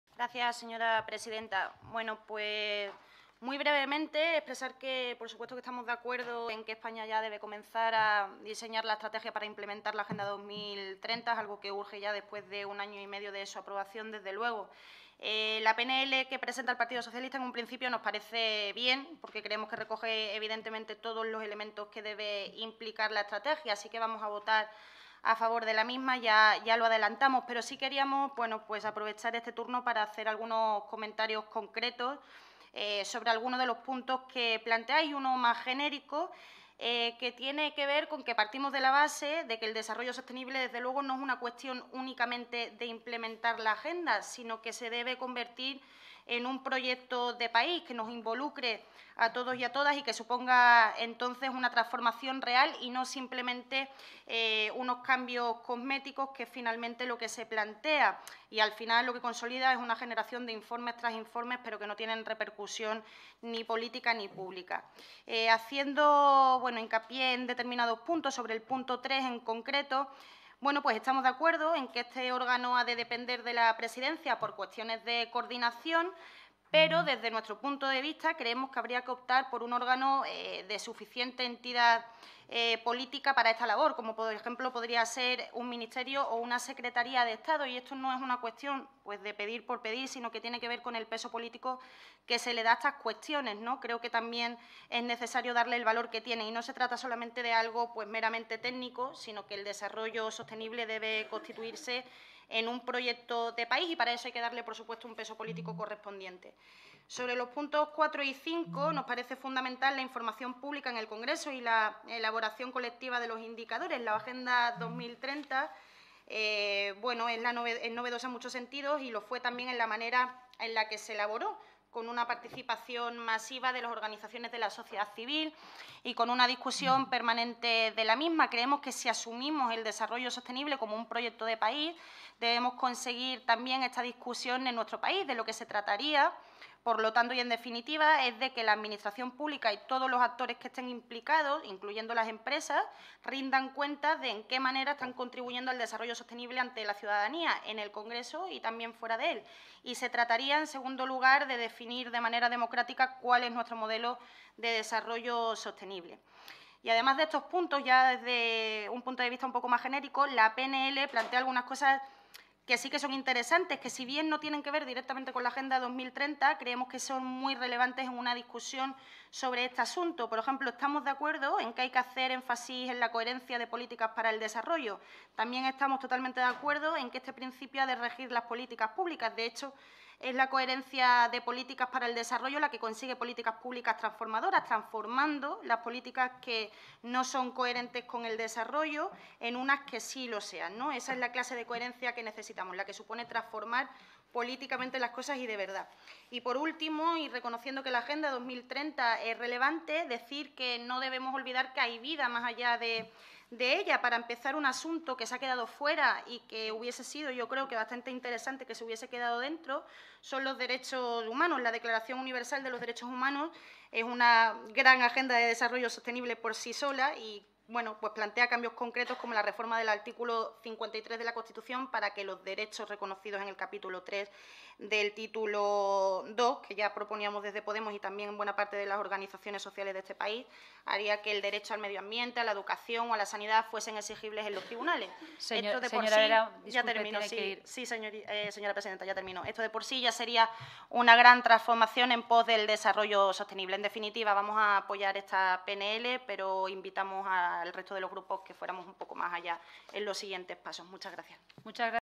Noelia Vera en la Comisión de Cooperación Internacional para el Desarrollo el 4 de Abril
Sobre la proposición no de Ley sobre la implementación en España de la Agenda 2030.